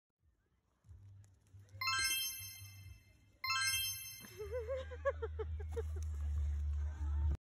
CUTE FUNNY SQUIRREL GRABS 2 NUTS ON THE PARK BENCH
Filmed in Brighton - ultra tame squirrels love monkey nuts! So cute and funny when they take two at once!